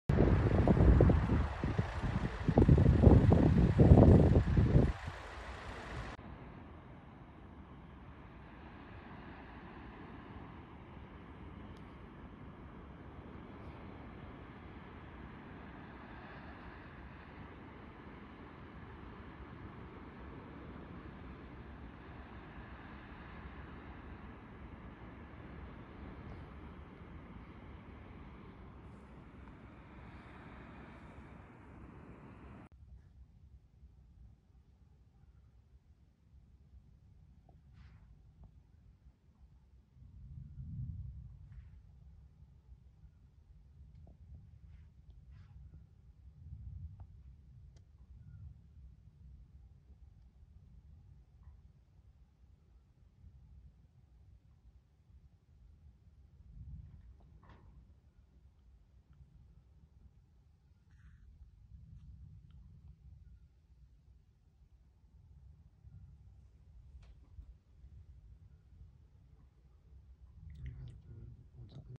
Spectacular Etna Volcanic Eruption || ViralHog
Occurred on February 22, 2021 / Taormina, Sicily, Italy

"This video has been recorded during Etna eruptions of February 2021 from iPhone 12 mini. Views are from Fiumefreddo and Taormina in Sicily"